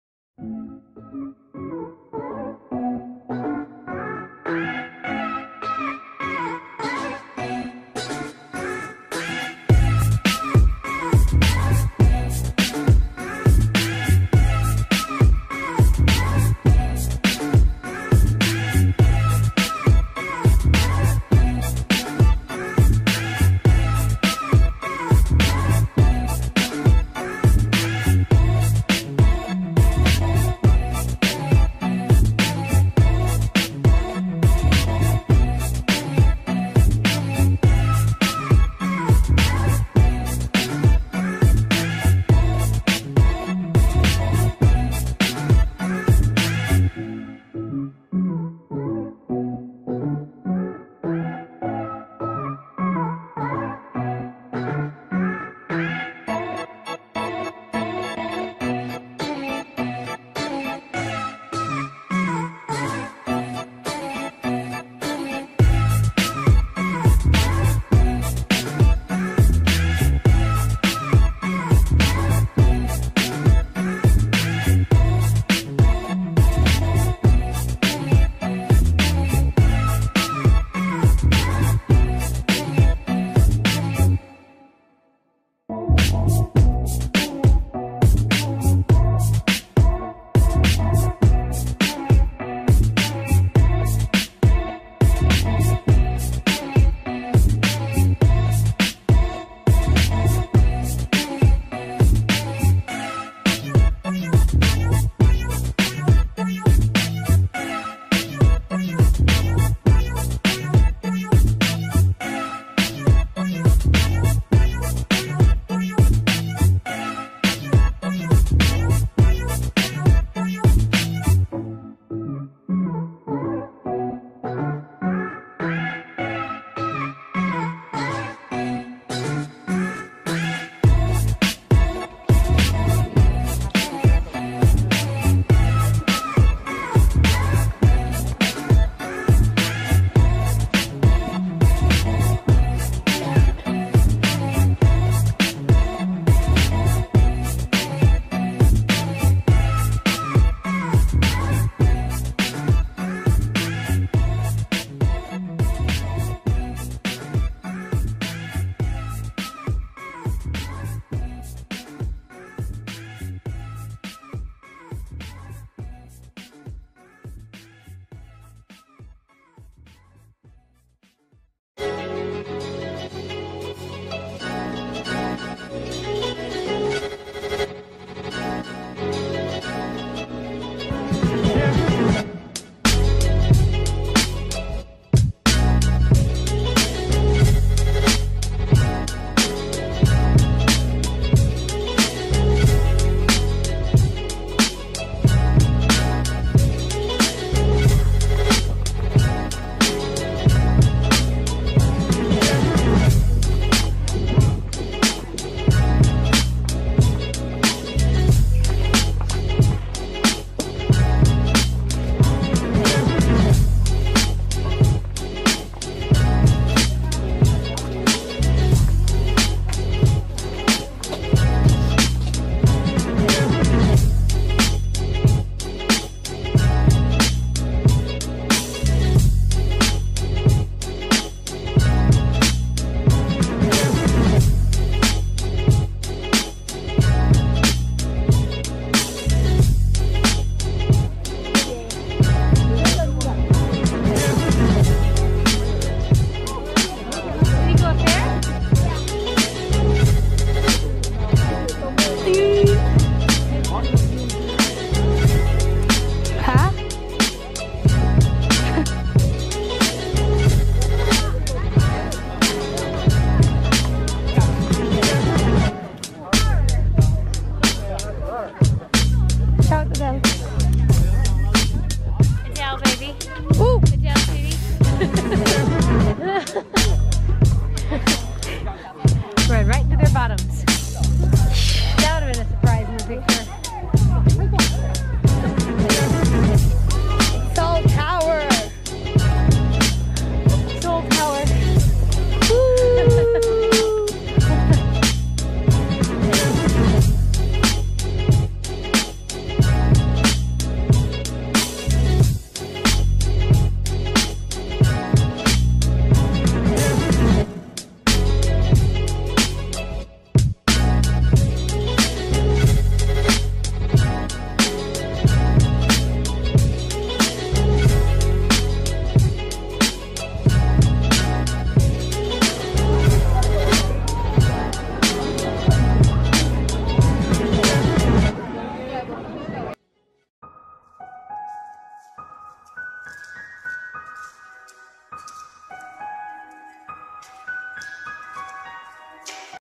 Hanok Village + Seoul Tower
Ancient Village in SEOUL, where there are still residence living there. There are scouts who try to remind visitors to respect the residence by being quiet and  not littering while walking around the place.

N Seoul Tower, also commonly known as Namsan Tower, is an iconic Seoul landmark that provides bird's-eye views of the city.